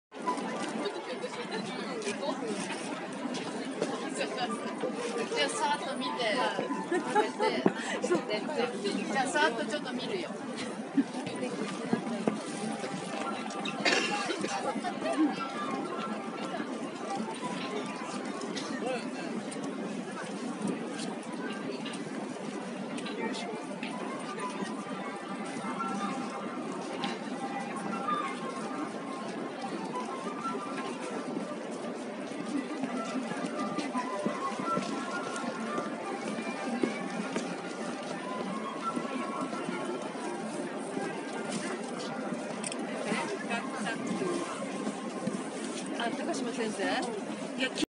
Tokyo Quilt Festival 2018
A few short videos from the International Tokyo Great Quilt Festival 2018